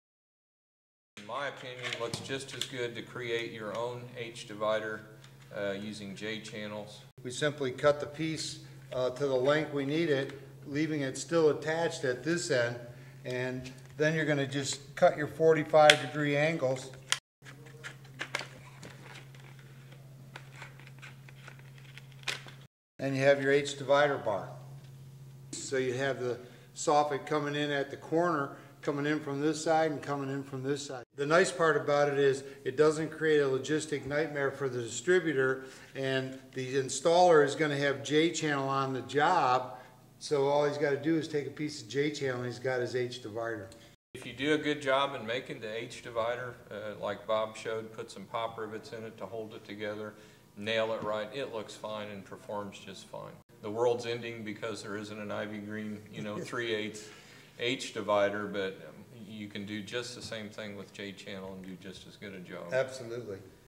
Bob Werner & JD Smith make H-Divider from Vinyl J-Channel - a Reese 59 Second Snapshot ytv200a
Vinyl Siding can transform the exterior of a home...but only if it's installed correctly. The Reese series "Installing Vinyl Siding" helps show you how.
In this video Bob Werner from CertainTeed and JD Smith from Reese Wholesale team to show how to make H-Divider for soffit using regular Vinyl J Channel
Video # ytv200a by John Reese and UV Concepts